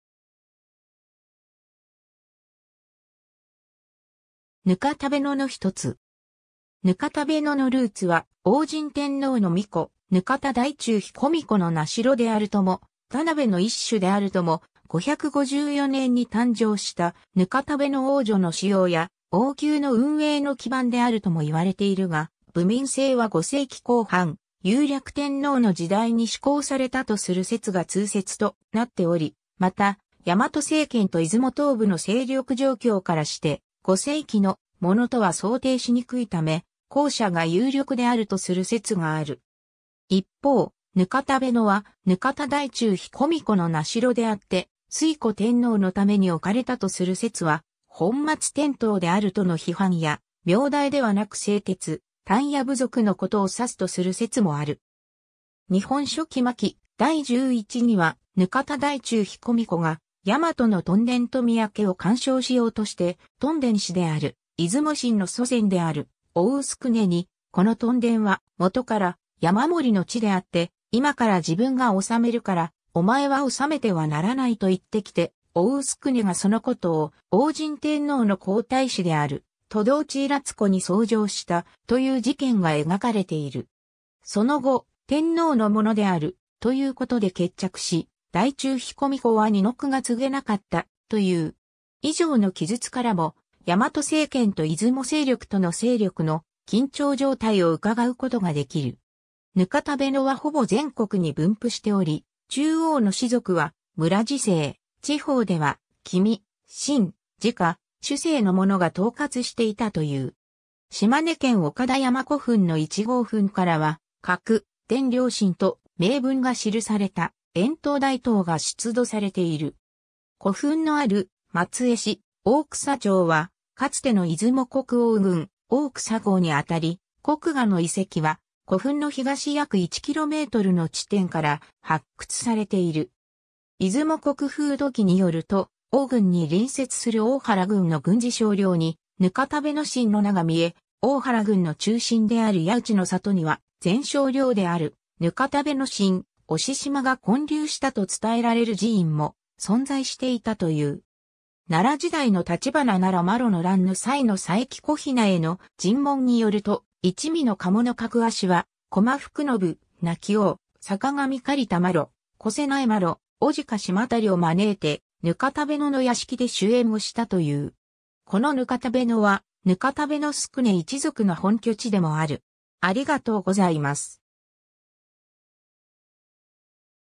額田部
額田部（ぬかたべ)とは、古代日本の部（名代）の一つ。
 額田部のルーツは応神天皇の皇子、額田大中彦皇子（ぬかたのおおなかつひこのみこ）の名代であるとも、田部の一種であるとも、554年（欽明天皇15年）に誕生した額田部皇女（のちの推古天皇）の資養や王宮の運営の基盤であるとも言われているが、部民制は5世紀後半、雄略天皇の時代に施行されたとする説が通説となっており、また大和政権と出雲東部の勢力情況からして、5世紀のものとは想定しにくいため、後者が有力であるとする説がある。
一方、額田部は額田大中彦皇子の名代であって、推古天皇のために置かれたとする説は本末転倒であるとの批判や、名代ではなく製鉄・鍛冶部族のことを指すとする説もある。
 『日本書紀』巻第十一には、 額田大中彦皇子が大和の屯田（みた）と屯倉を管掌しようとして、屯田司である出雲臣の祖先である淤宇宿禰（おうのすくね）に、「この屯田はもとから山守の地であって、今から自分が治めるから、お前は治めてはならない」と言ってきて、淤宇宿禰がそのことを応神天皇の皇太子である菟道稚郎子に奏上した、という事件が描かれている。
その後、天皇のものである、ということで決着し、大中彦皇子は二の句が継げなかった、という。
以上の記述からも、大和政権と出雲勢力との勢力の緊張状態を伺うことができる。
 額田部はほぼ全国に分布しており、中央の氏族は連姓、地方では、君・臣・直・首姓のものが統轄していたという。
島根県岡田山古墳の1号墳からは「各田了臣」と銘文が記された円頭大刀が出土されている。
古墳のある松江市大草町はかつての出雲国意宇郡大草郷にあたり、国衙の遺跡は、古墳の東約1キロメートルの地点から発掘されている。
『出雲国風土記』によると、意宇郡に隣接する大原郡の郡司少領に「額田部臣」の名が見え、大原郡の中心である屋裏（やうら）の郷（現在の雲南市）には、前少領である額田部臣押嶋（ぬかたべ の おみ おししま）が建立したと伝えられる寺院も存在していたという。
 奈良時代の橘奈良麻呂の乱の際の佐伯古比奈への尋問によると、一味の賀茂角足は、高麗福信、奈貴王、坂上苅田麻呂、巨勢苗麿、牡鹿嶋足を招いて、（現在の大和郡山市と比定される）「額田部」の屋敷で酒宴をしたという。
この額田部は額田部宿禰一族の本拠地でもある。